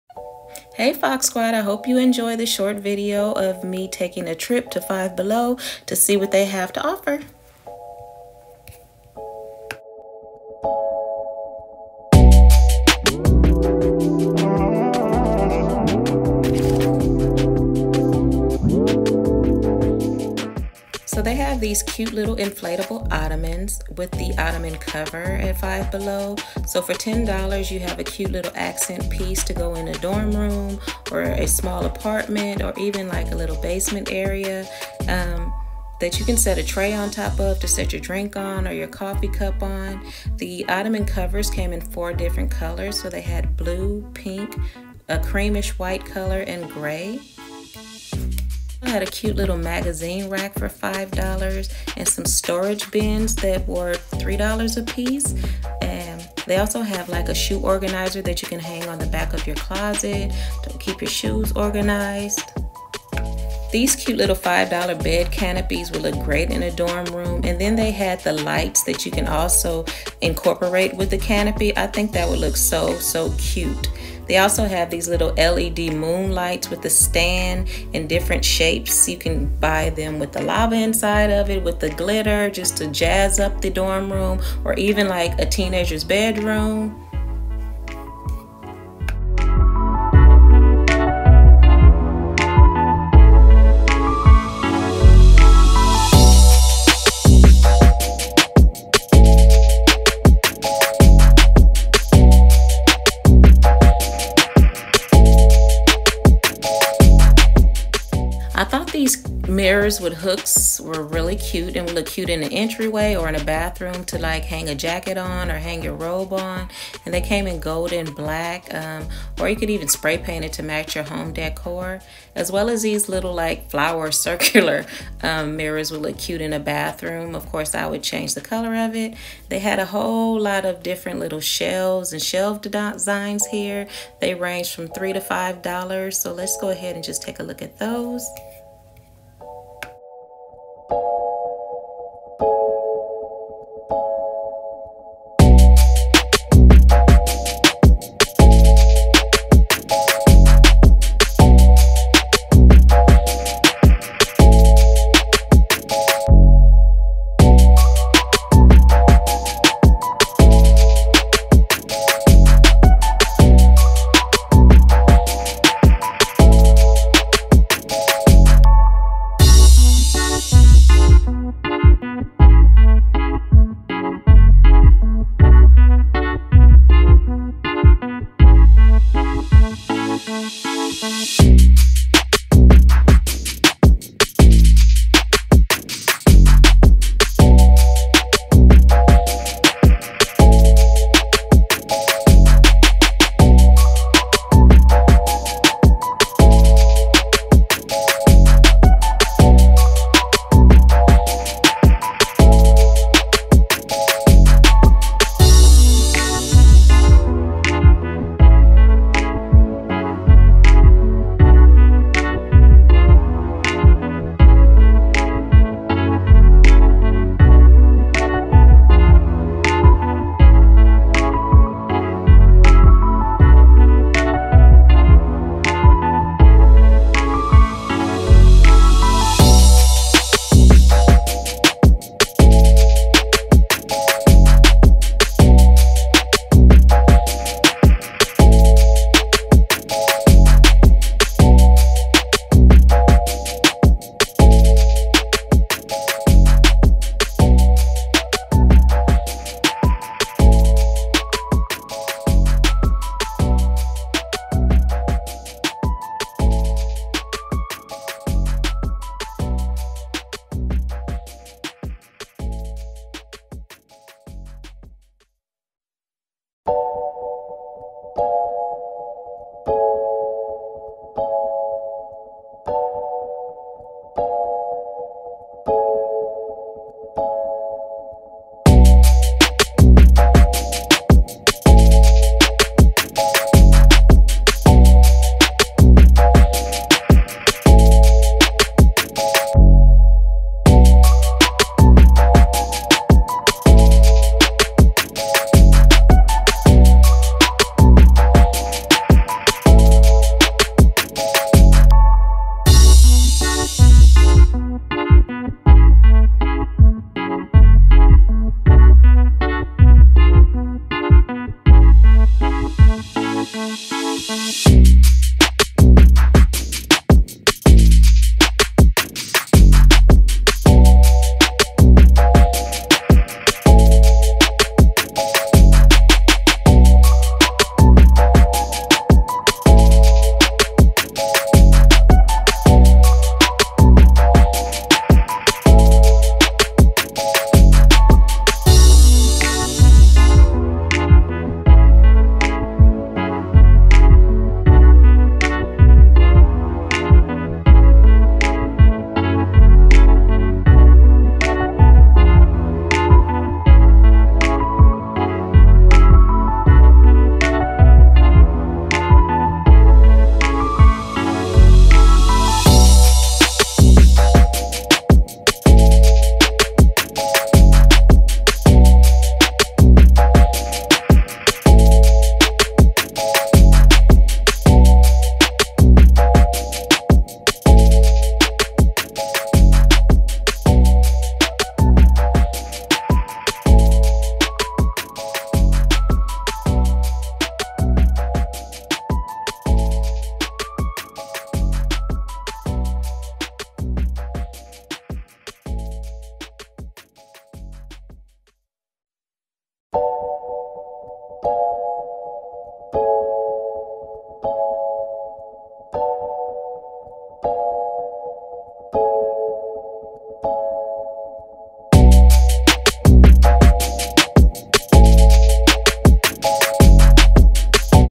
WHY YOU SHOULD SHOP @ FIVE BELOW | AFFORDABLE HOME, DORM and OFFICE DECOR 😊
Hello Foxx Squad!  I took a trip to Five Below to check out the extremely affordable home decor and more they have to offer.

A few items you may be searching for that are available at Five Below.
Faux plants
Decorative Shelves
Accent Pillows
Accent Rugs
Throws
Faux Fur Pillow Covers
Inflatable Ottoman & Ottoman Covers
Leather Rugs
Workout Equipment